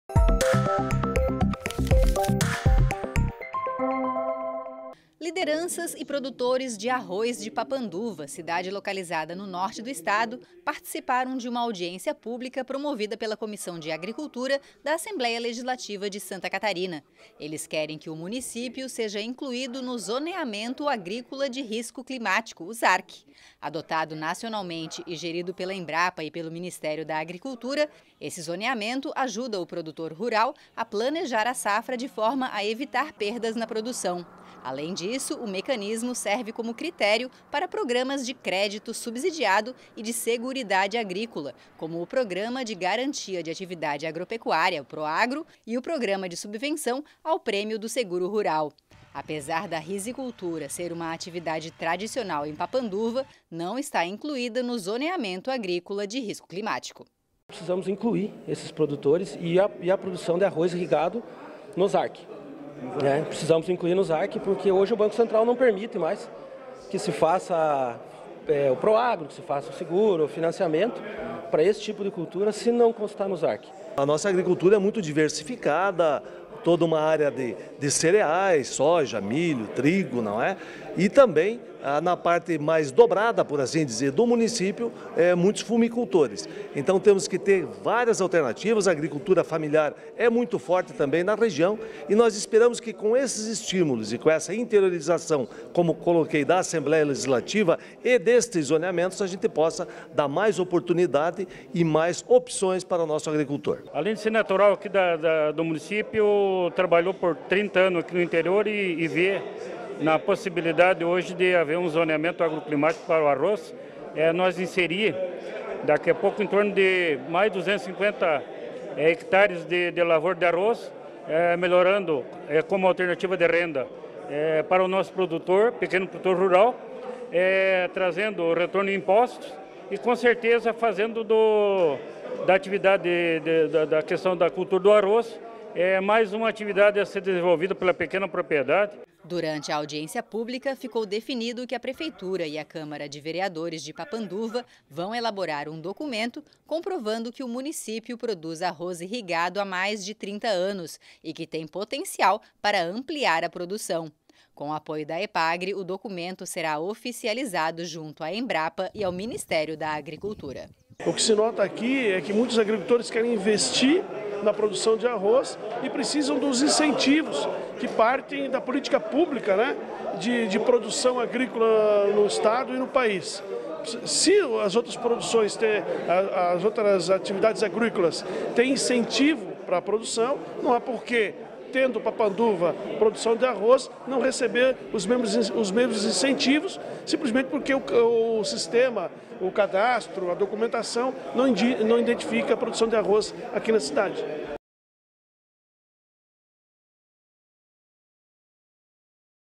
É Notícia - Rizicultores de Papanduva querem inclusão em zoneamento agrícola - 31/03/22
Lideranças e produtores de arroz de Papanduva, cidade localizada no norte do Estado, participaram de uma audiência pública promovida pela Assembleia Legislativa de Santa Catarina. Eles querem que o município seja incluído no Zoneamento Agrícola de Risco Climático (Zarc), adotado nacionalmente e gerido pela Embrapa e pelo Ministério da Agricultura.